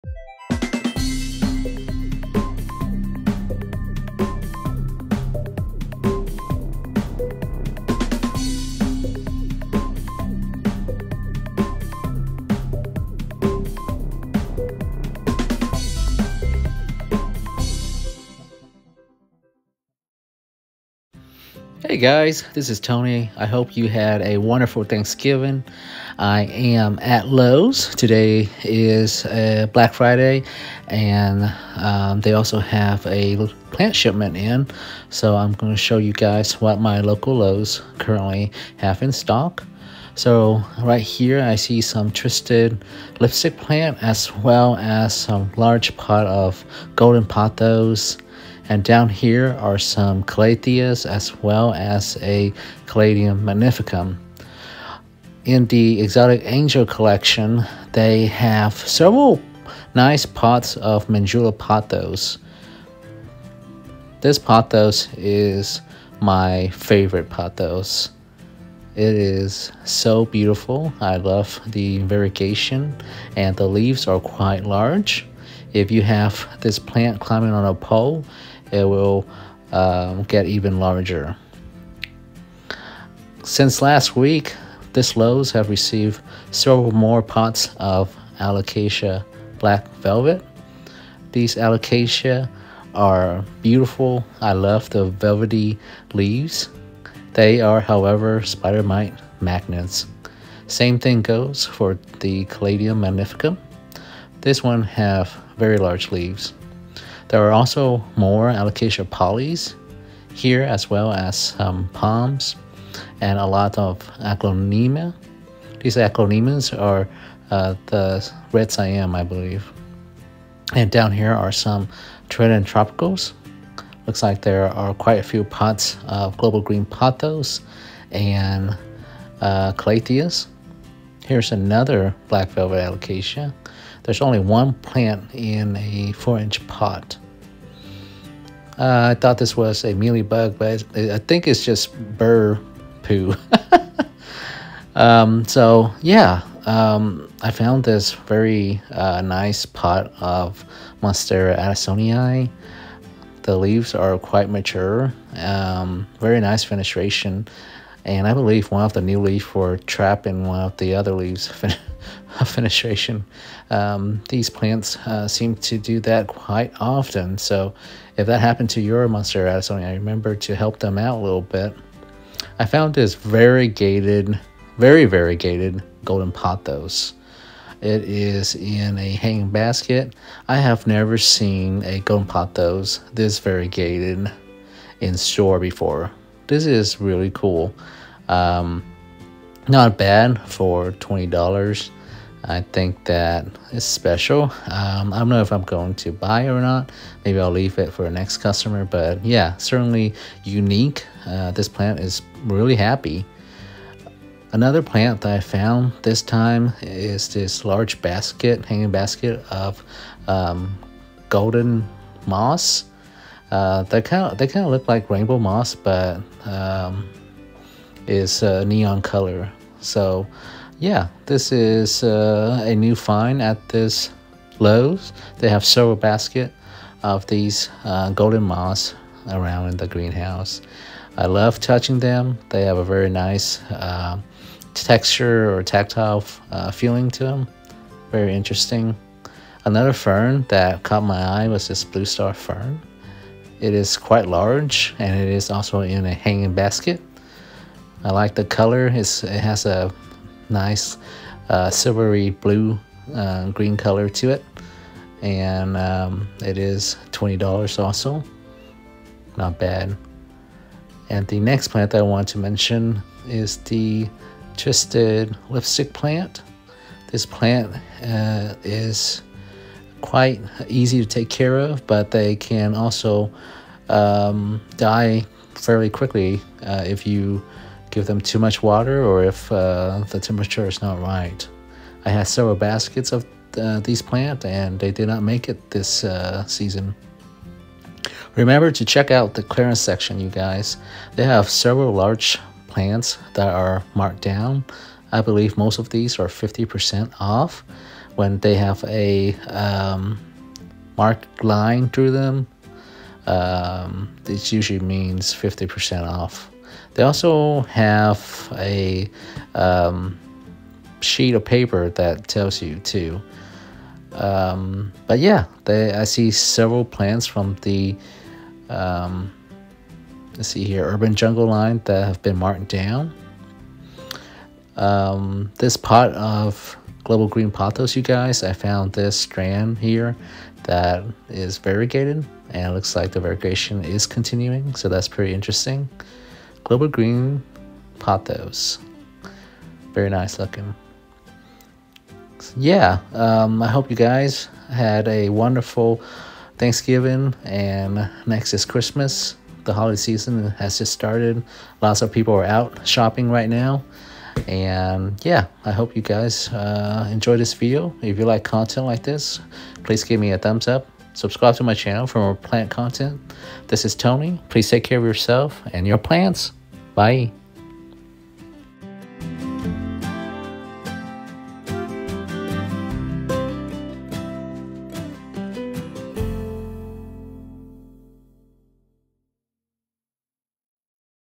Plant Shopping Lowe’s on Black Friday
I found some nice pots of Manjula pothos, Golden Moss today a Lowe’s, and variegated global green pothos.